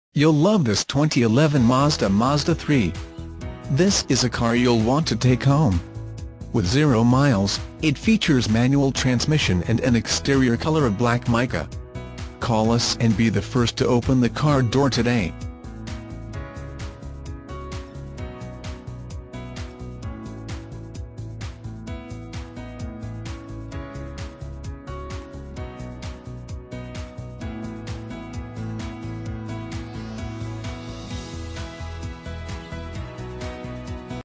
2011 MAZDA MAZDA3 Salinas, CA Z18791
2011 MAZDA MAZDA3 Salinas, CA
Stock

Cardinale Mazda (Salinas)
500 Autocenter

You'll love this 2011 Mazda MAZDA3. This is a car you'll want to take home. With 0 miles, it features manual transmission and an exterior color of Black Mica. Call us and be the first to open the car door today!